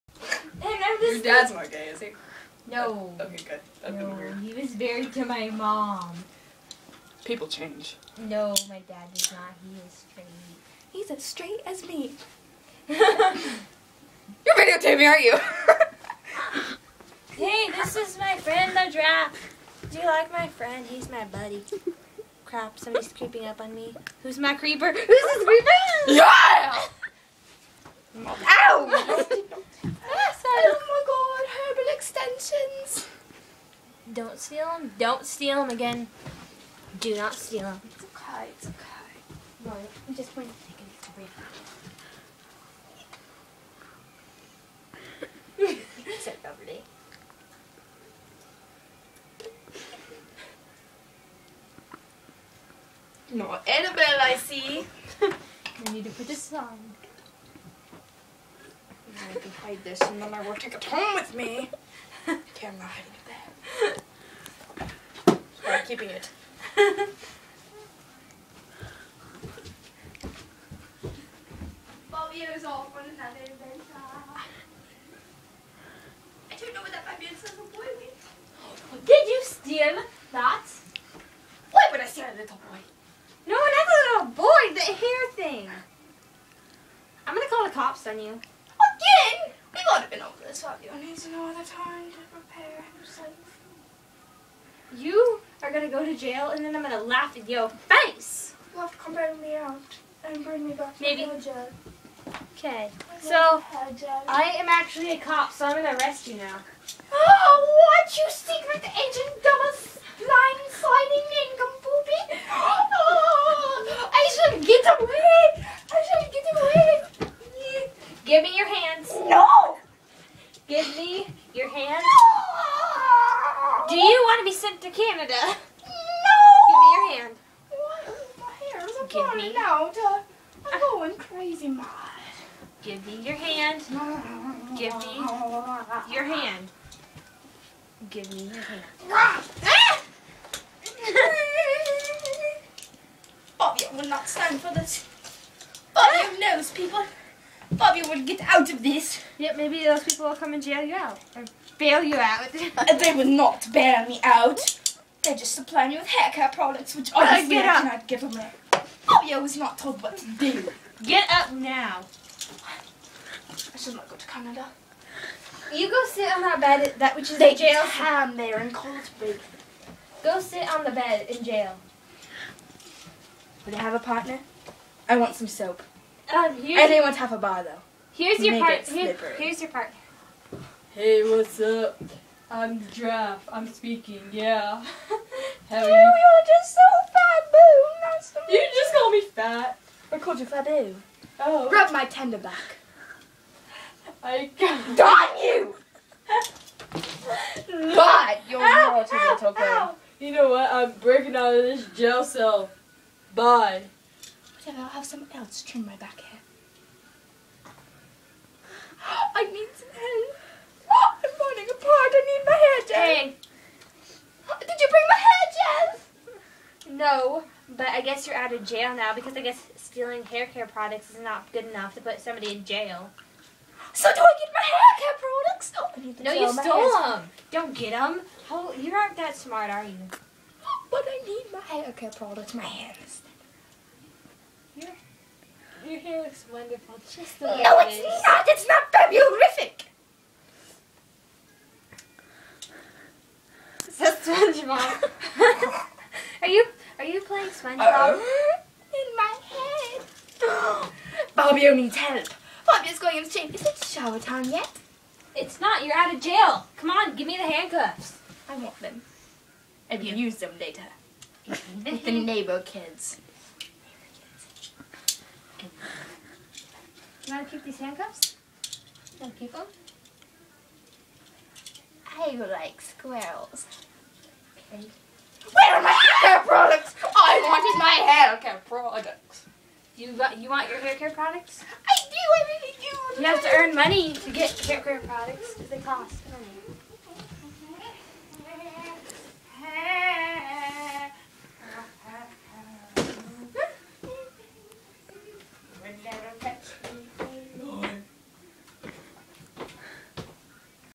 Fabio & Friends: Fabio Gets Arrested
Fabio gets arrested for stealing hair products then gets a strange cell mate and wants to keep the hand cuffs so he can catch those dang neighbor kids and turn them in!